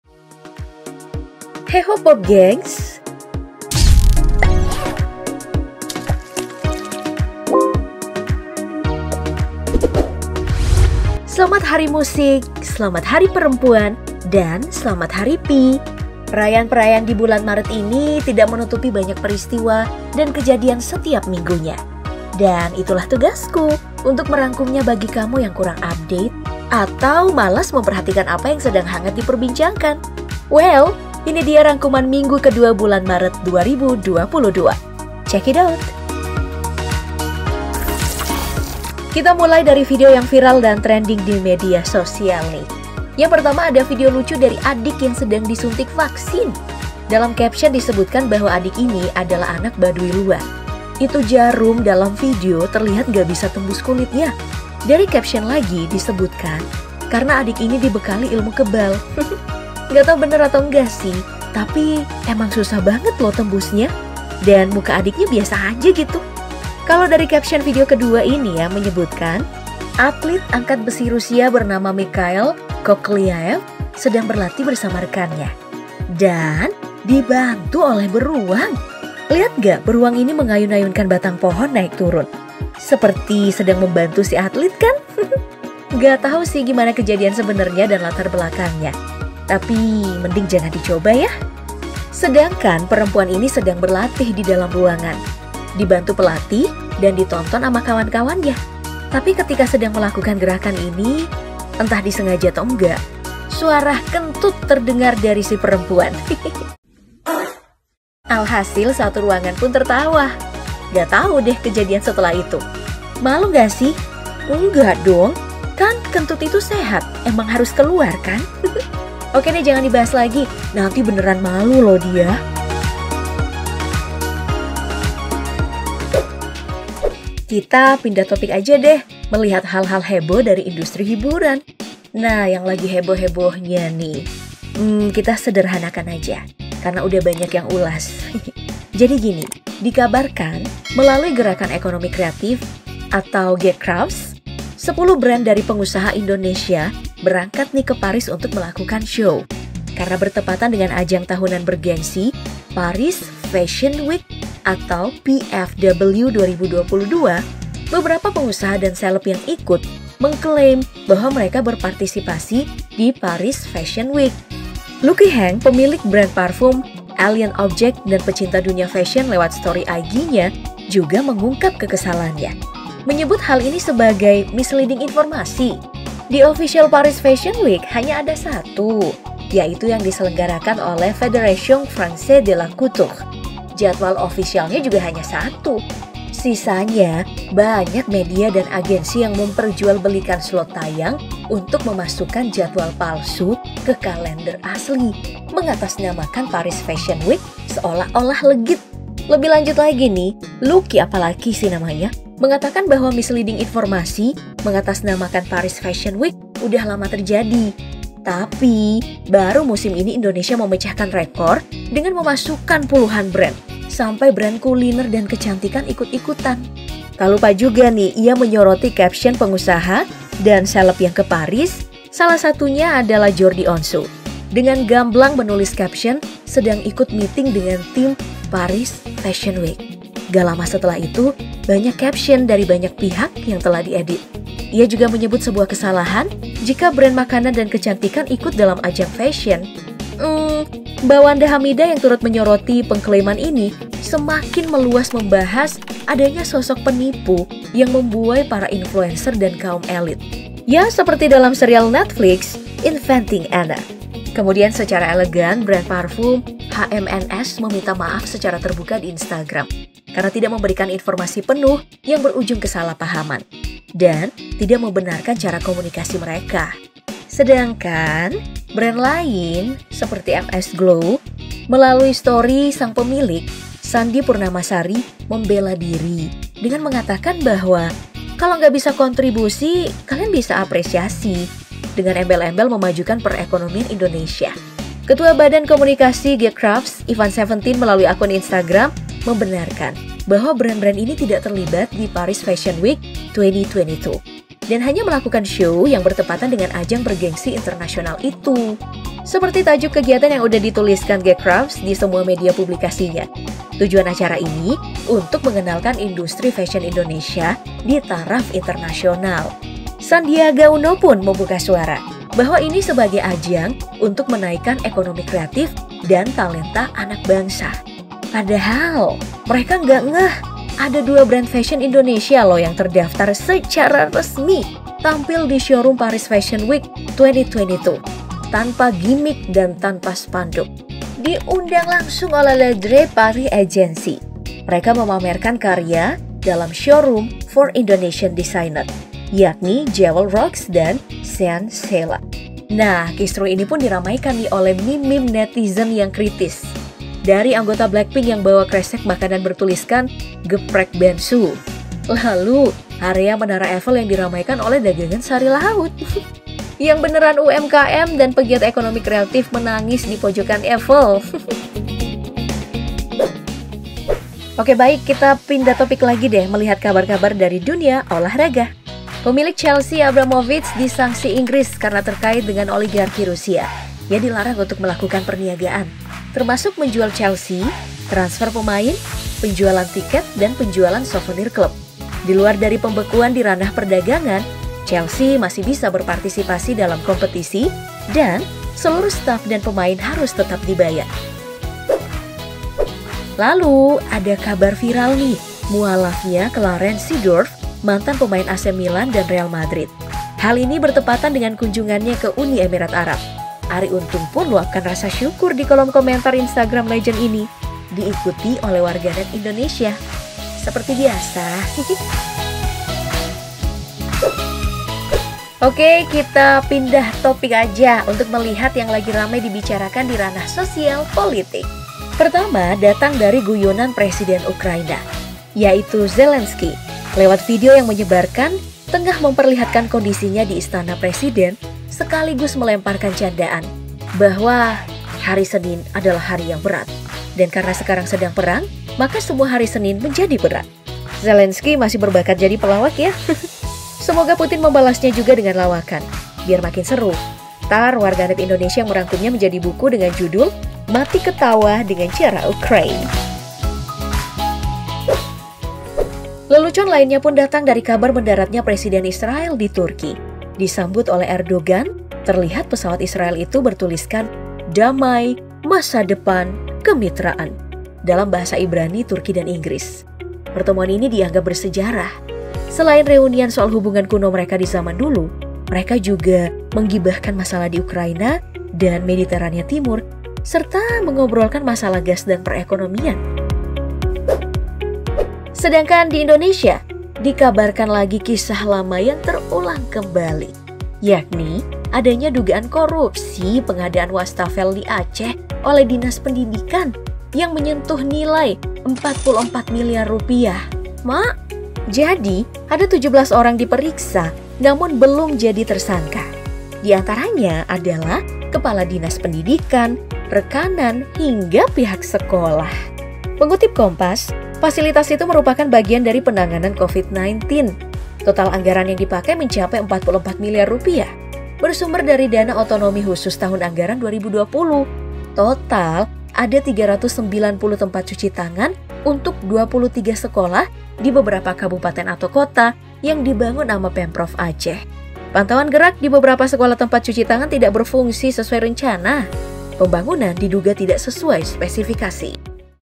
Heboh! Latihan Keras Orang² Rusia Bersama Beruang, Hal Aneh Terjadi di Gym dan Video Viral Lainnya
Selamat hari musik, selamat hari perempuan, dan selamat hari Pi. Perayaan-perayaan di bulan Maret ini tidak menutupi banyak peristiwa dan kejadian setiap minggunya. Dan itulah tugasku untuk merangkumnya bagi kamu yang kurang update atau malas memperhatikan apa yang sedang hangat diperbincangkan. Well, ini dia rangkuman minggu kedua bulan Maret 2022. Cek it out